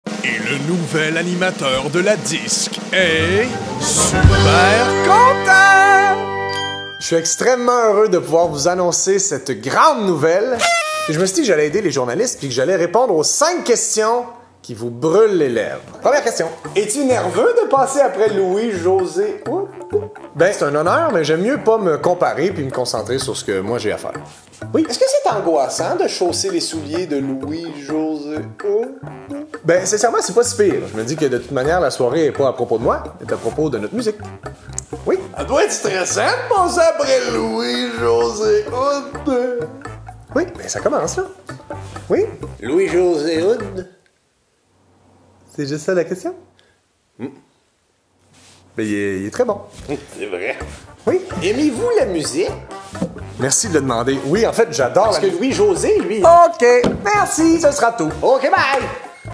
Pierre-Yves Roy-Desmarais remplace Louis-José Houde | Gala de l'ADISQ
L'humoriste Pierre-Yves Roy-Desmarais prendra le relais de Louis-José Houde à l'animation du 46e Gala de l'ADISQ, l'automne prochain.